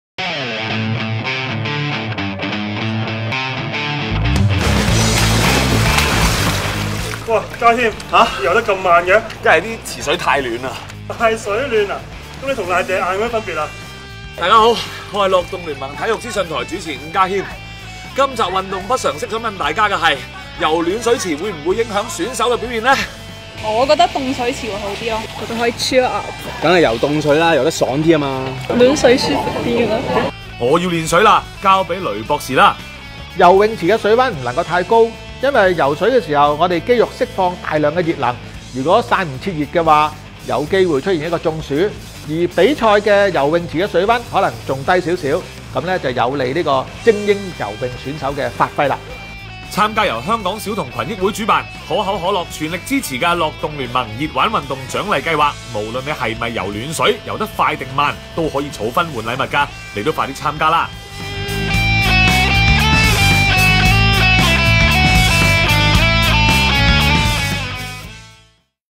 【樂動聯盟體育資訊台 - 暖水池會影響選手表現 ？】
身邊有班朋友好鍾意游水，無論冬夏，風雨不改。
早兩個月咪好鬼凍嘅？佢哋，竟然可以照游！我問唔凍㗎咩？「我哋游凍水先醒神呀！」其中一個仲話喺暖水池游得慢啲。
關事㗎咩？唔知呢方面雷博士有冇研究呢。